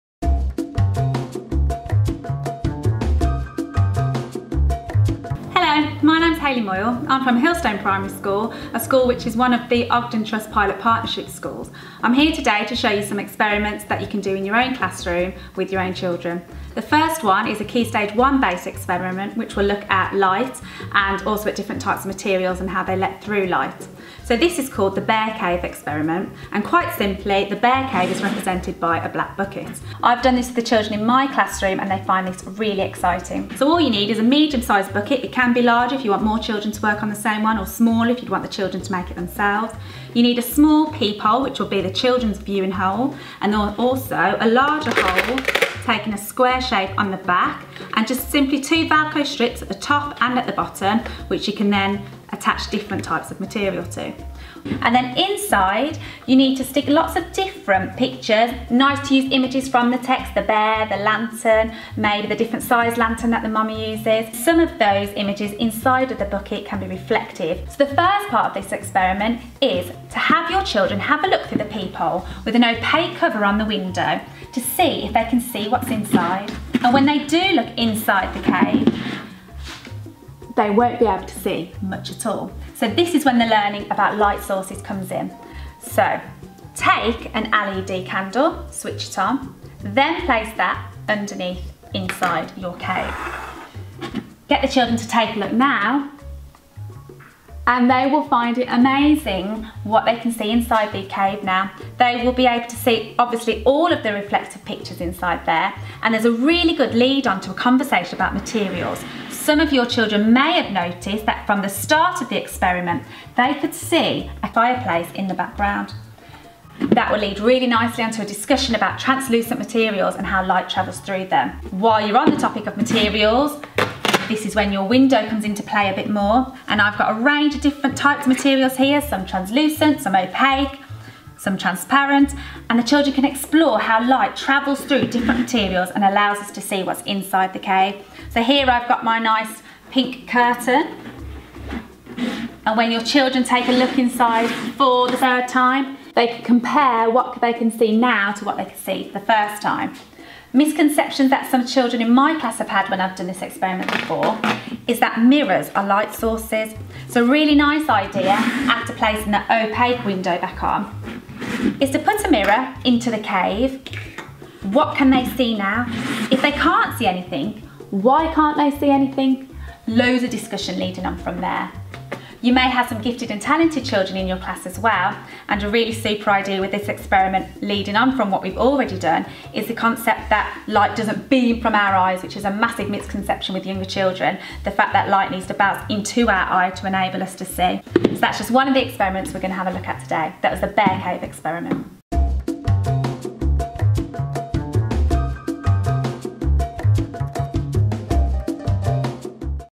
Primary Light & Sound: bear cave
A KS1 experiment exploring light and materials, using Can't You Sleep, Little Bear? as a literacy link to this science practical.

The Ogden bear cave phizzi practical resource sheet can be found on our website with a range of other primary curriculum resources